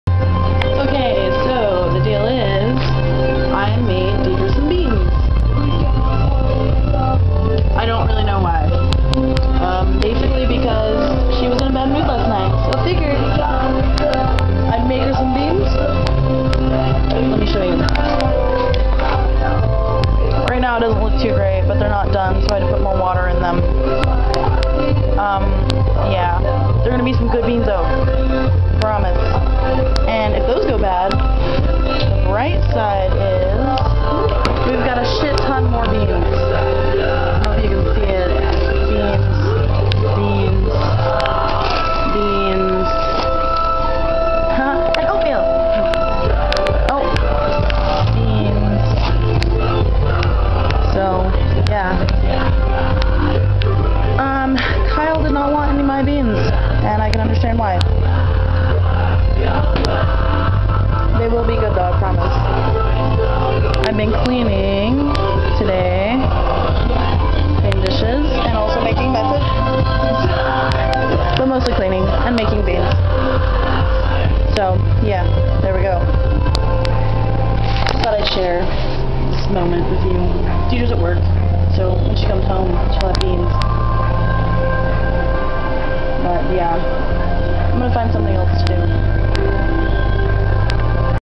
Beans, Sorrow, and MAGICK!
A rivetting tale abouut how one young girl tries (and fails) to cheer her friend up with Beans. magical beans! that means really fucking salty